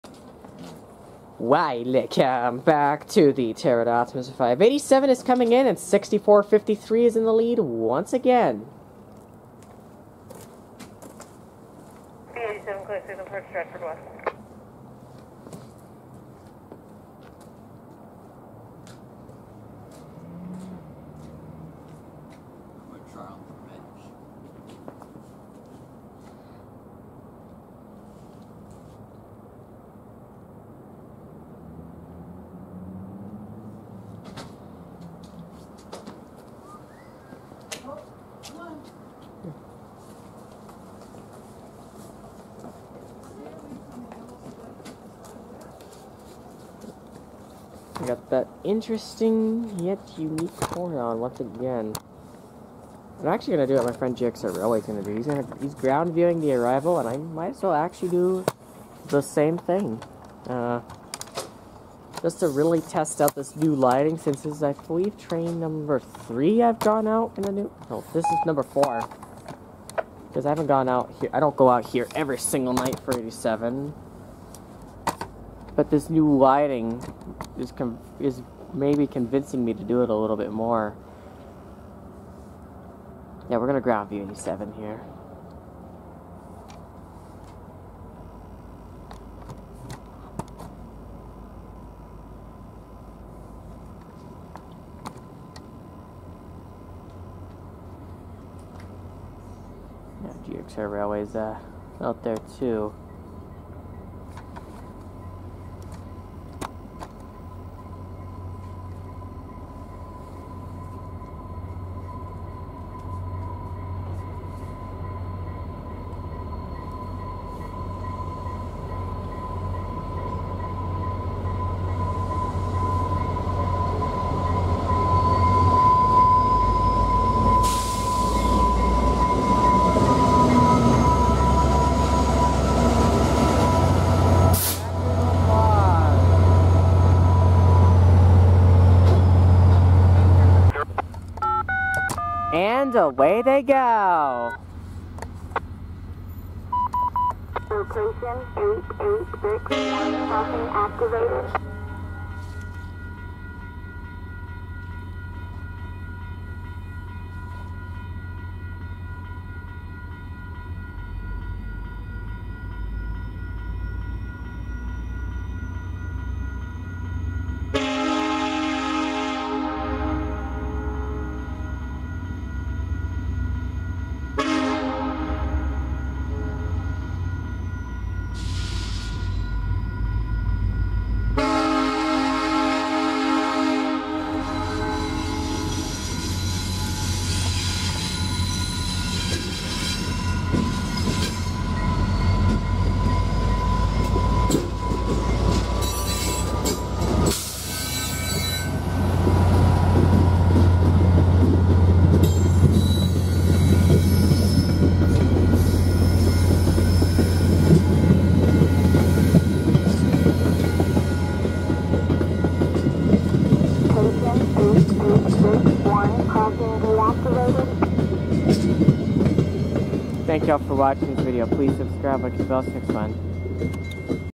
VIA 87 December 7, 2023
Train ID: VIA 87
Locomotive Number(s): 6453
Coach Numbers: 3459, 3311, 3308, 3356
Time: 20:01 EST
Location: Stratford VIA Station
Subdivision: Guelph
Milepost: 88.5
Origin: Toronto Union Station
Destination: Sarnia VIA Station